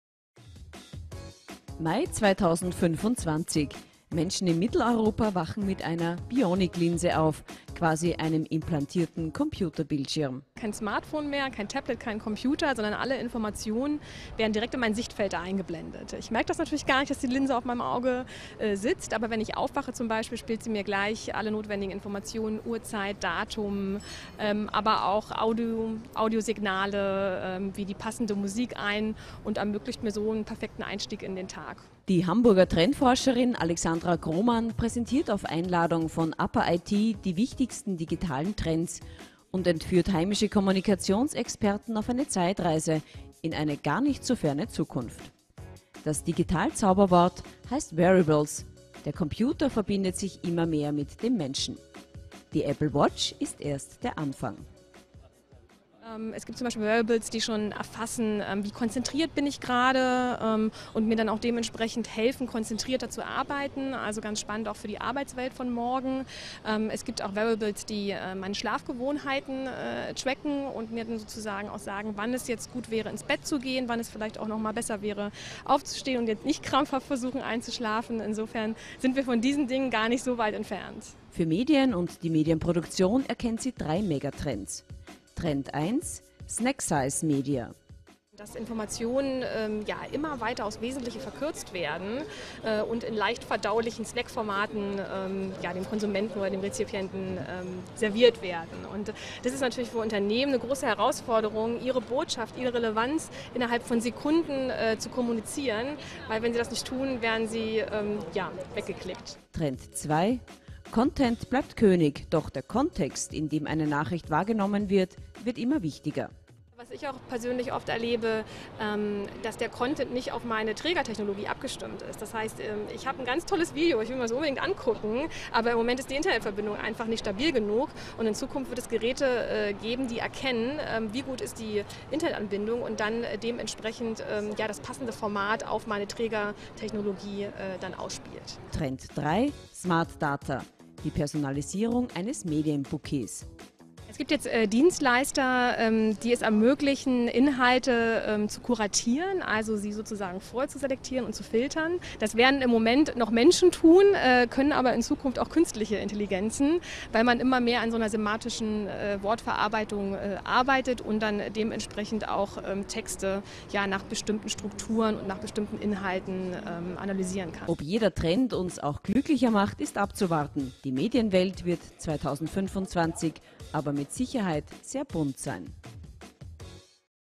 Jahresrückblick: Futuretrends - Die Welt 2025
MAI: Das Zauberwort in der Zukunft wird Wearables heißen, der Computer verbindet sich immer mehr mit den Menschen. Trendforscherin Alexandra Grohmann wagt einen Ausblick.